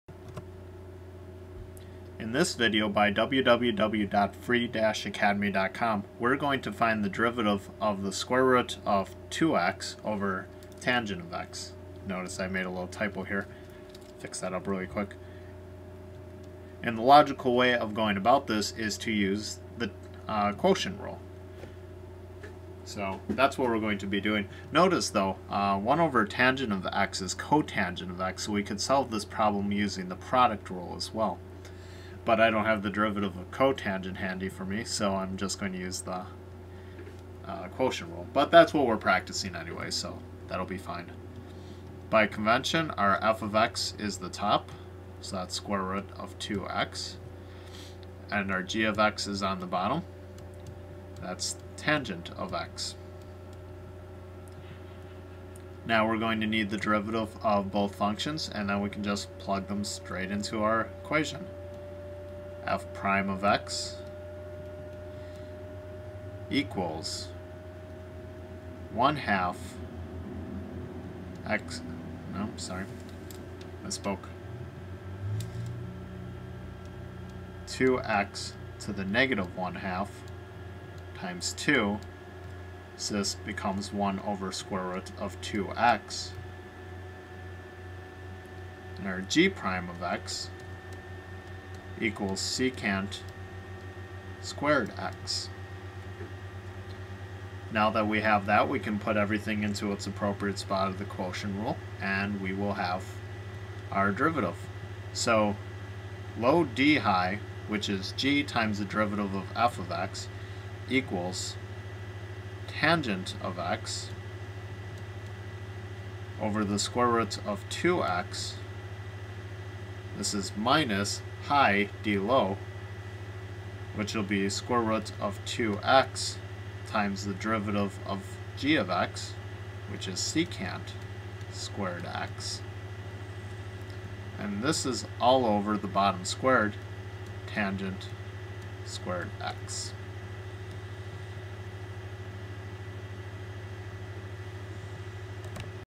Quotient Rule Practice Problem 2 - sqrt(2x)*tan x - Calculus
Free Calculus Lecture practicing the Quotient Rule by finding the derivative of sqrt(2x)*tan x. Presented